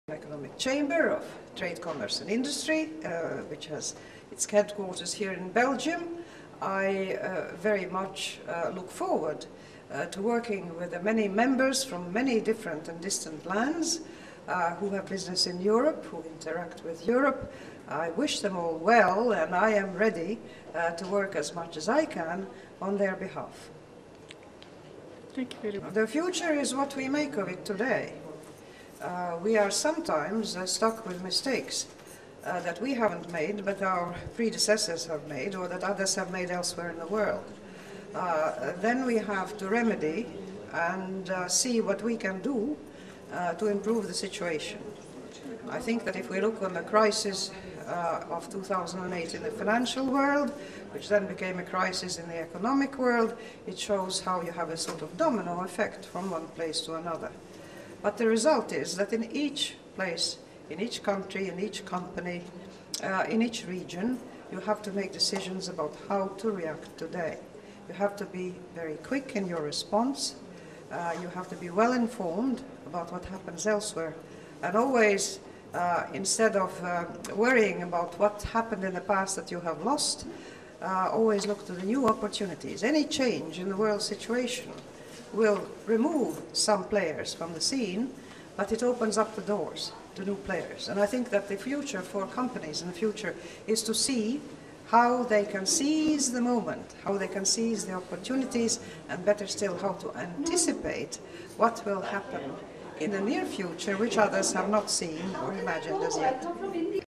Interview with Mrs. Vaira Vike Freiberga, President of Organization EEIG
Meeting of the  EEIG Board of Directors and 4th President's Conference,6-7 June 2014, Interview with Mrs  Vaira Vike Freiberga, President of Organization EEIG.